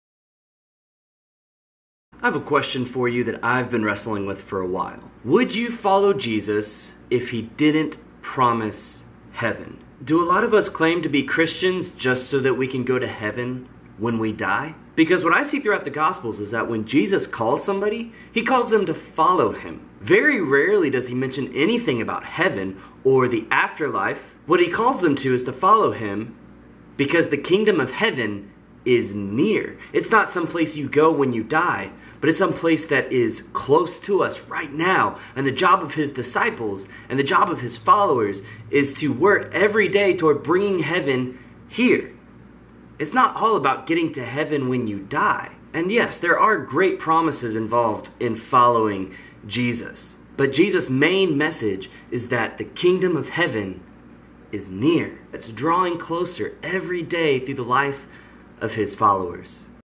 Discipleship Minute: The Kingdom of Heaven
(September 2, 2016) What is the kingdom of heaven all about? Following Jesus means seeking the Kingdom of God right here and now, not just waiting for heaven when we die.